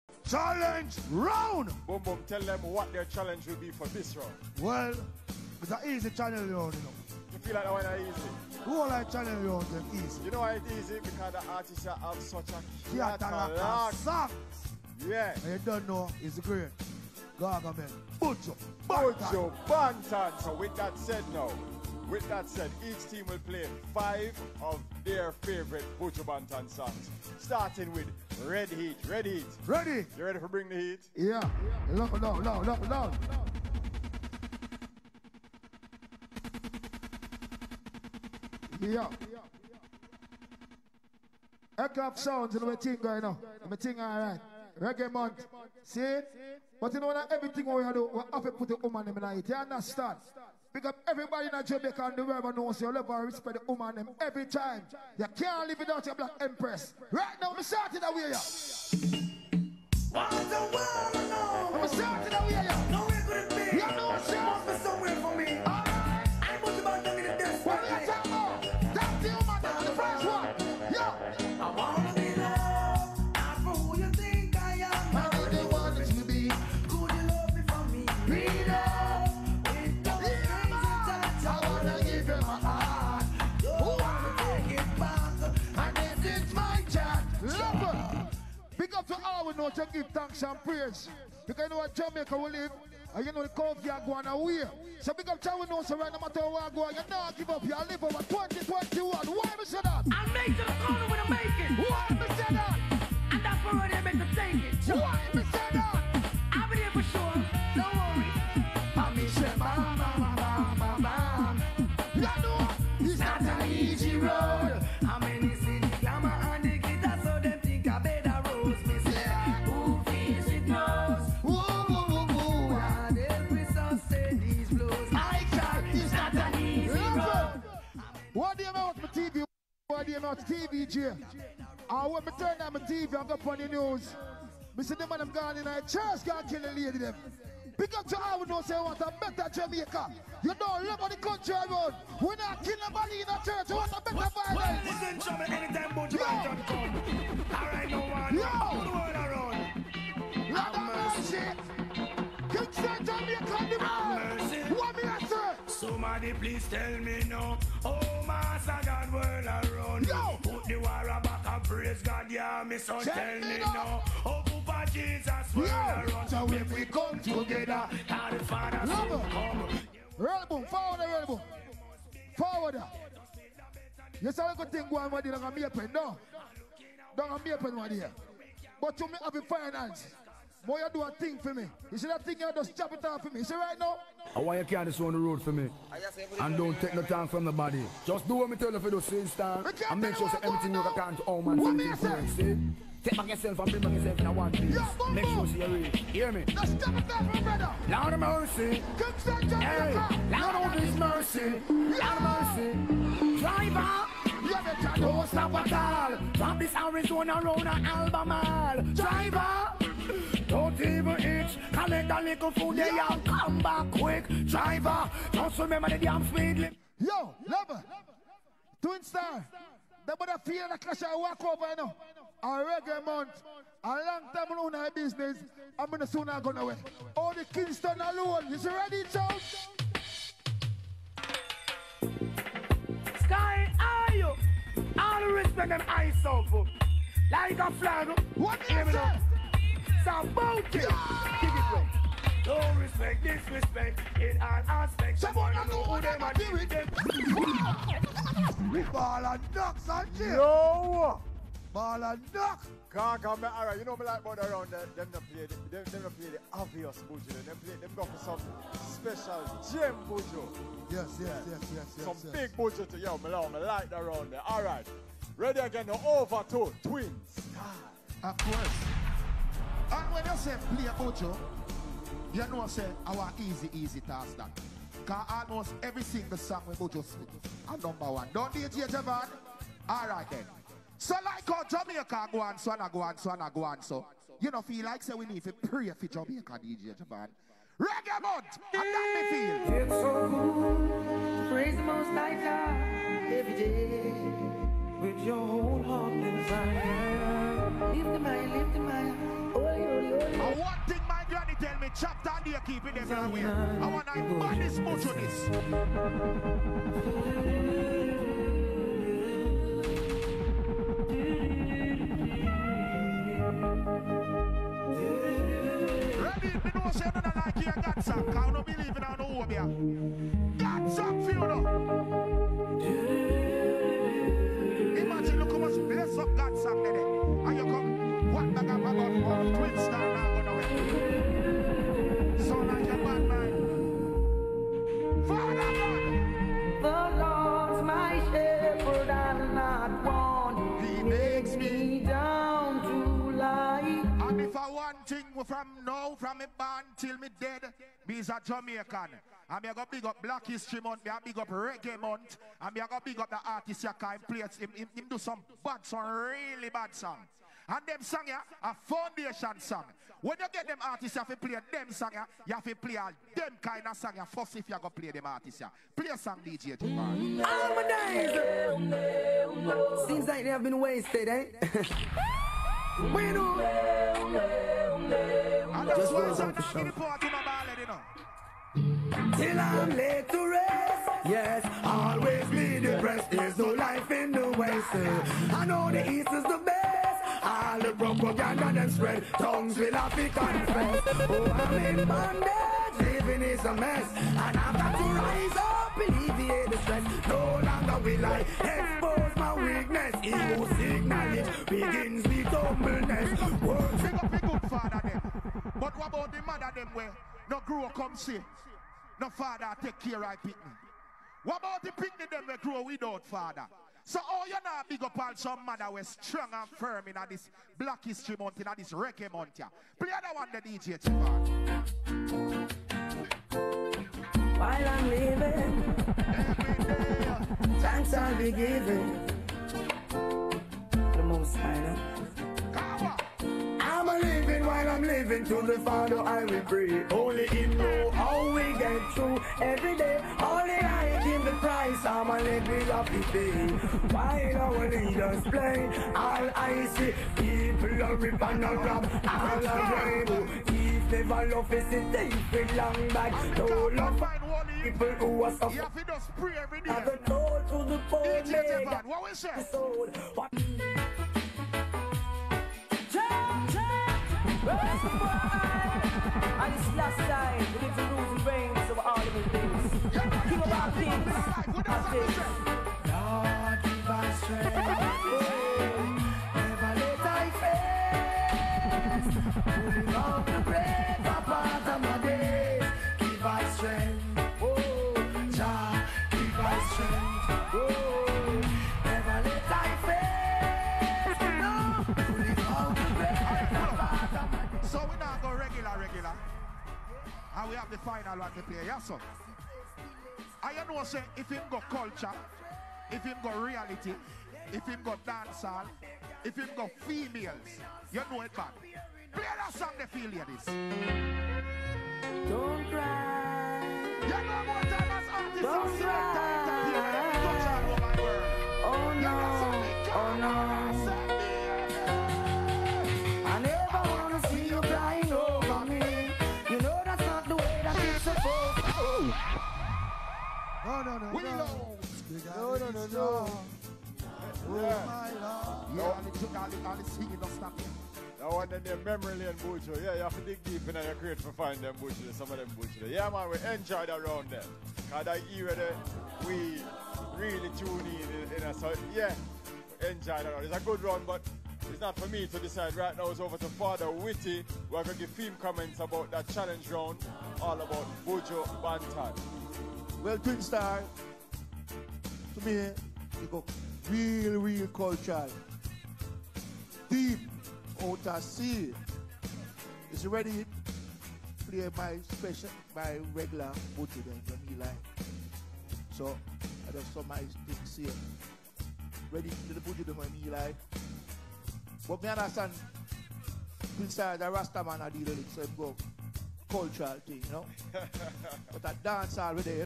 REDD HEAT vs TWIN STAR, Echoes of Sound Systems Clash #2021, Challenge Round
Ministry of Culture, Gender, Entertainment & Sports.
In collaboration with The Ministry of Tourism, Tourism Entertainment Fund, CHASE, Jamaica Tourist Board, PBC Jamaica & Reggae Month 2021.
Presents the final showdown (Challenege Round - Buju Bantan) Echoes of Sound Systems Clash 2021 Semi Finals
Showing their technics, skills and ability to entertain the judges and the public.
Judges on the night, Admiral Bailey, Father Whitty & Squeeze.
Hosts on the night, Marlon"Boom Boom"Wizard & Sanjay Ramanand.